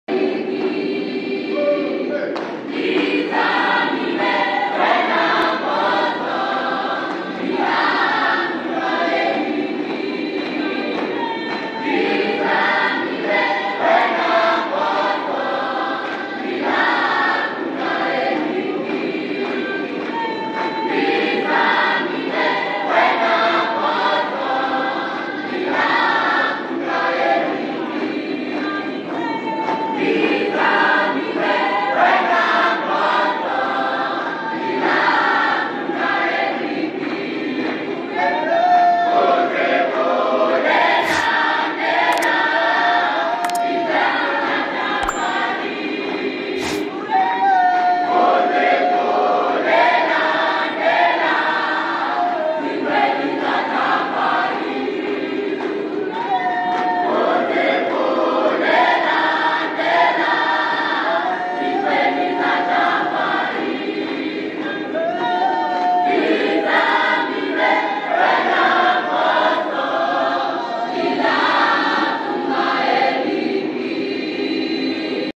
OAC Juventude de Jafar- Inauguração da capela de sikwama
OAC Juventude Geral de Jafar.
Celebração alusivo a inauguração da capela de Malhapseni sikwama. Foi sensacional a actuação da Juventude Geral de Jafar que fez no altar de sikwama.
Muita força a juventude geral de jafar, nos abençoe sempre com vossas canções celestiais💪.

Quer conhecer a origem da OAC?
Preço: 355 Mts.